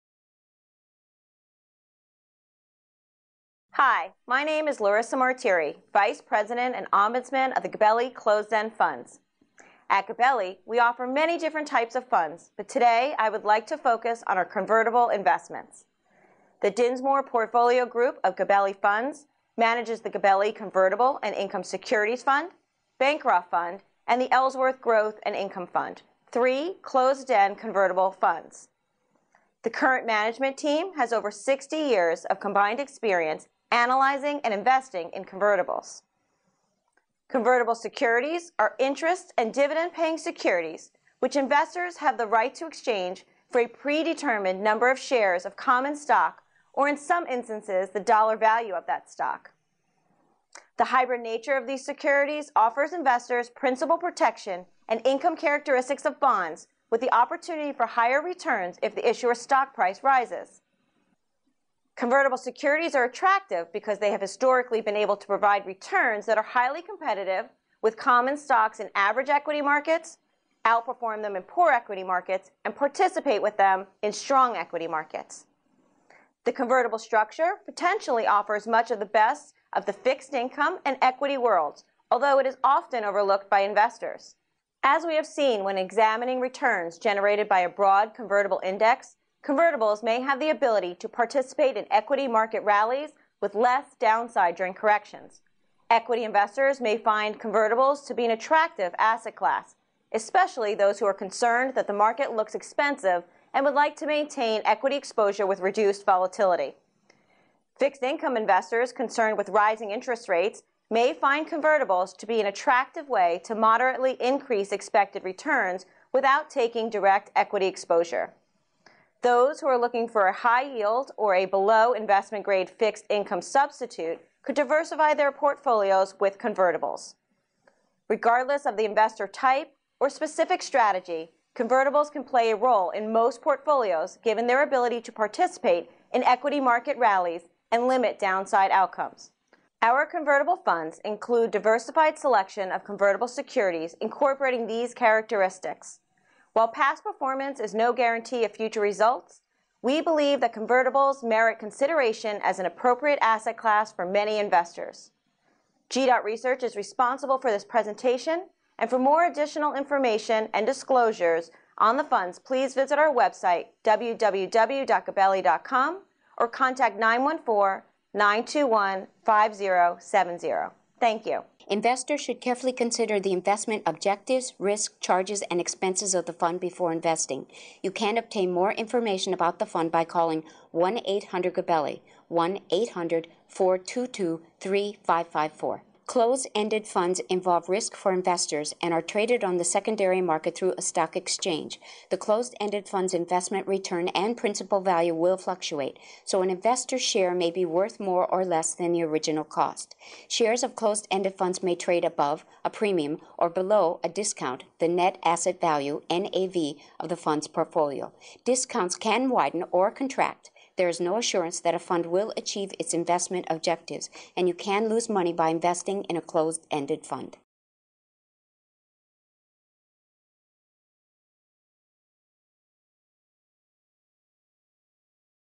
Convertible Investments - Gabelli Vice President & Ombudsman Laurissa Martire
GabelliTV

Transcript:My name is Laurissa Martire, Vice President and Ombudsman of the Gabelli Closed-End Funds.  At Gabelli we offer many different types of funds, but today I would like to focus on our convertible investments.
The Dinsmore Portfolio Group of Gabelli Funds manages The Gabelli Convertible and Income Securities Fund, Bancroft Fund, and Ellsworth Growth and Income Fund, three closed-end convertible funds. The current management team has over 60 years of combined experience analyzing and investing in convertibles.
Convertible securities are interest and dividend paying securities which investors have the right to exchange for a predetermined number of shares of common stock, or, in some instances, the dollar value of that stock.
The hybrid nature of the securities offers investors the principal protection and income characteristics of bonds with the opportunity for higher returns if the issuer’s stock price rises.
Convertible securities are attractive because they historically have been able to provide returns that are highly competitive with common stocks in average equity markets, outperform them in poor equity markets and participate with them in strong equity markets.
The convertible structure potentially offers much of the best of both the fixed income and equity worlds, although it is often overlooked by investors.
As we have seen when examining the returns generated by a broad convertible index, convertibles may have the ability to participate in equity market rallies with less downside during corrections.
Equity investors may find convertibles to be an attractive asset class, especially those who are concerned that the market looks expensive and would like to maintain equity exposure with    reduced volatility.
Fixed income investors concerned with rising interest rates may find convertibles to be an attractive way to moderately increase expected returns without taking direct equity exposure.
Those who are looking for a high yield or a below investment grade fixed income substitute could diversify their portfolios with convertibles.
Regardless of the investor type or the specific strategy, convertibles can play a role in most portfolios given their ability to participate in equity market rallies and limit downside outcomes. Our convertible funds include a diversified selection of convertible securities incorporating these characteristics.
While past performance is no guarantee of future results, we believe that convertibles merit consideration as an appropriate asset class for many investors.